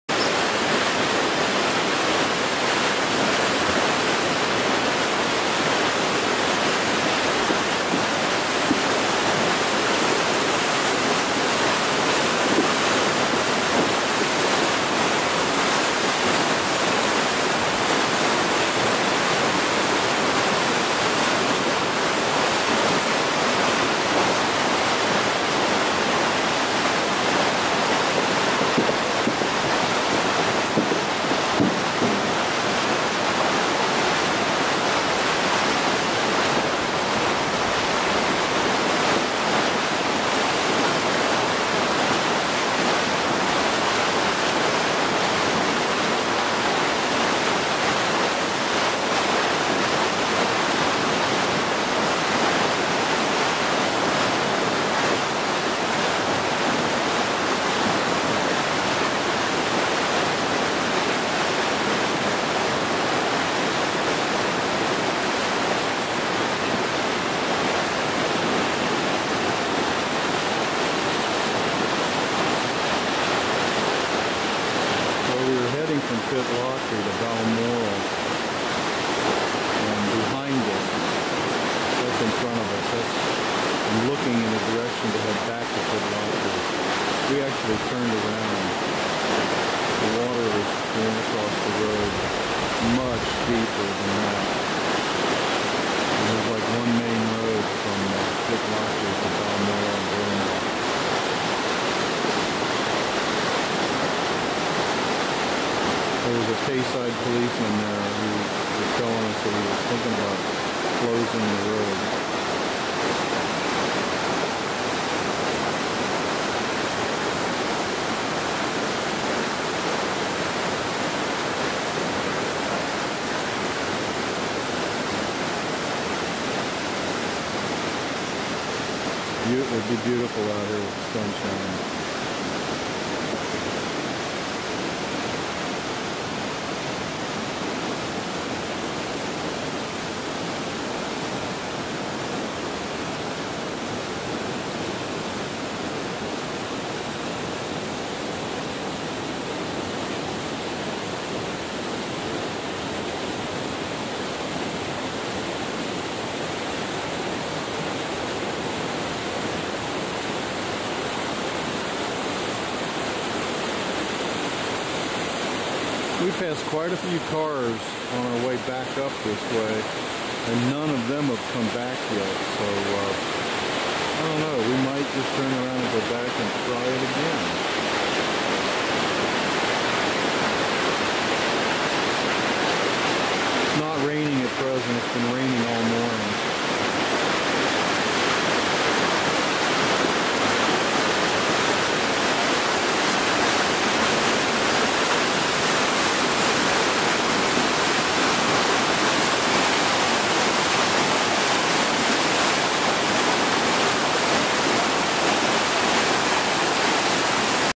A924 Pitlochry to Balmoral 21-Jul-2010
A924 from Pitlochry to Balmoral - 21-Jul-2010
High water after two days of rain.
Failed attempt to go to Balmoral Castle